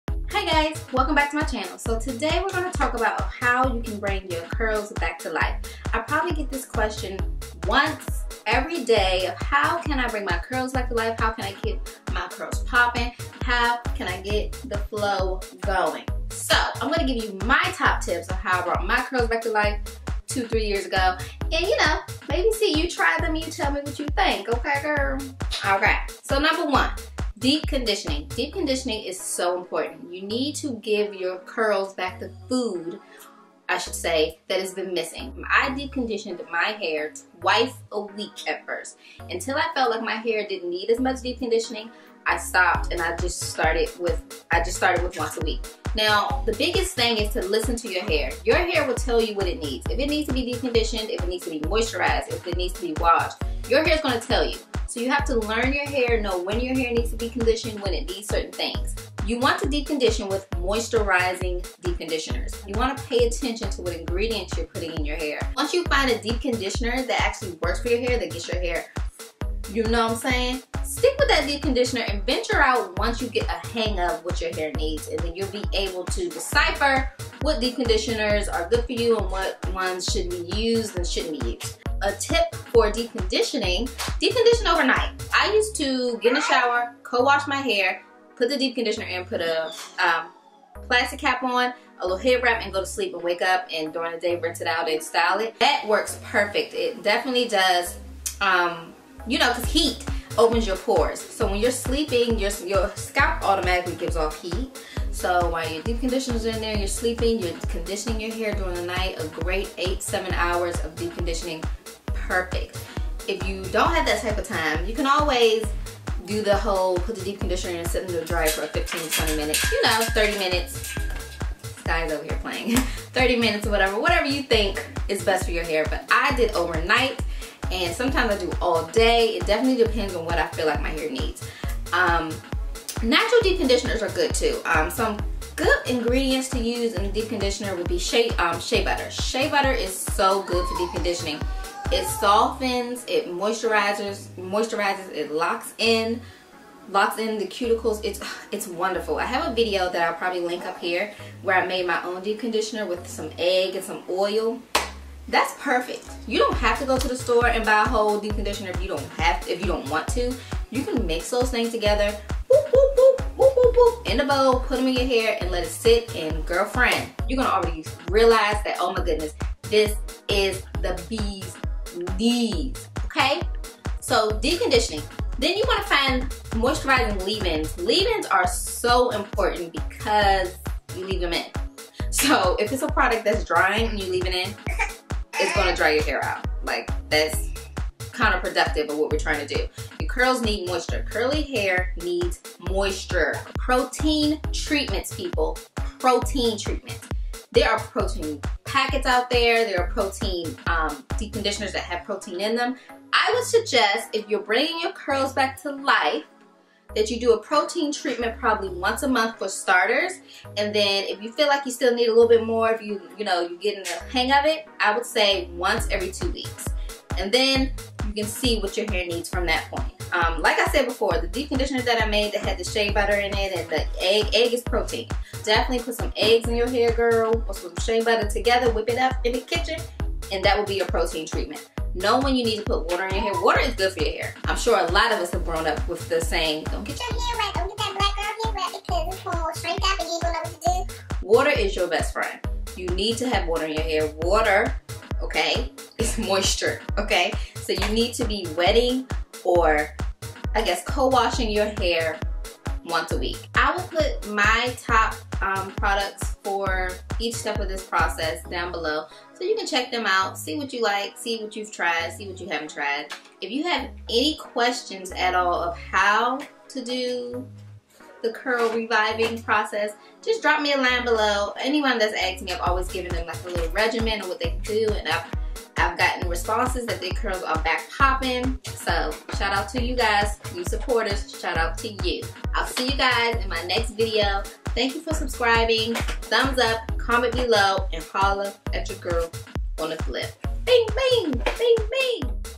How to Revive Your Curls | Deep Conditioning, Protein Treatments, etc
Hey Curlies!

I'm always getting asked how to revive & reboot your curls, Here are some tips on how it can be done! Enjoy!

My Favorite 'REBOOT' Products

* TGIN Honey Miracle Mask

Leave In's

* Miss Jesse's Leave In Condish